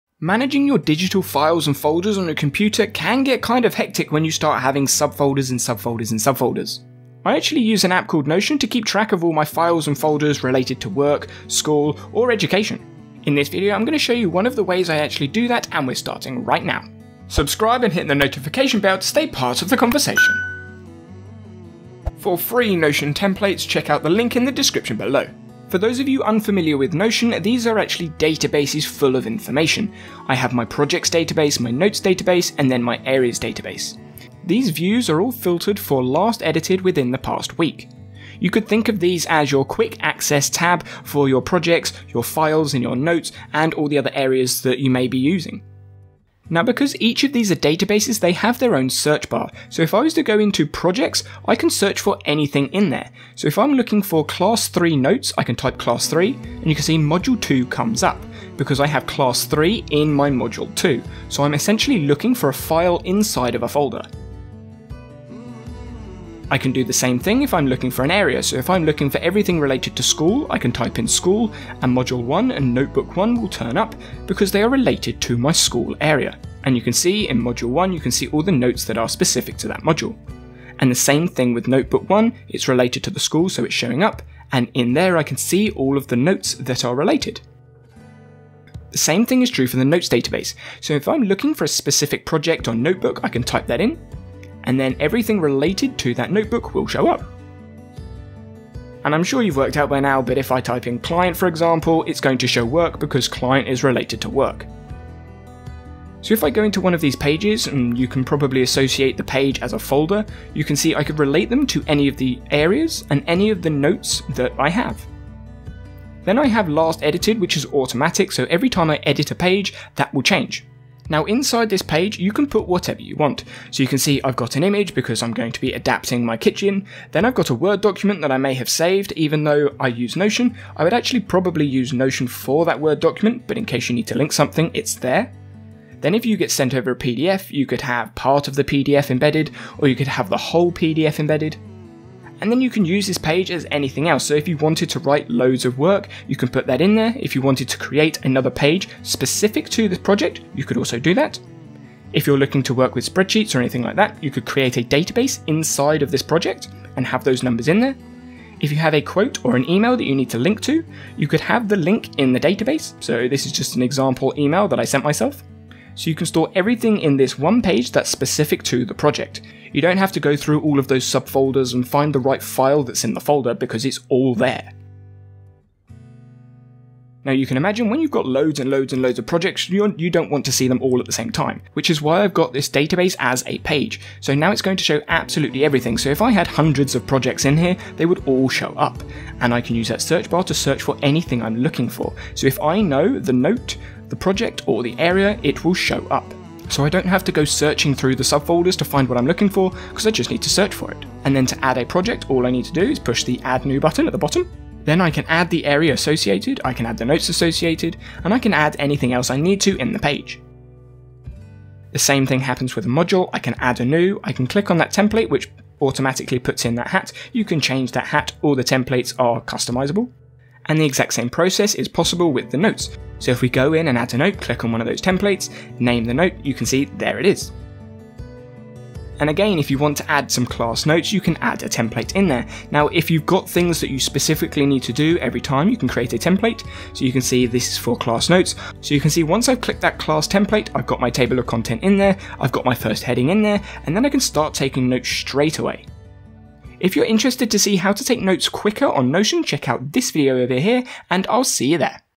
Manage Files And Folders EASILY in Notion
Knowing how to manage files and folders and perform basic file operations in Notion can speed up your workflow. To manage files on an iPad or computer can be a challenging task but if they are saved in Notion organization can become much cleaner.

⏰ TIMESTAMPS ⏰
0:00 Intro
0:25 Explanation
1:50 Pages
2:35 Email
3:04 Folder search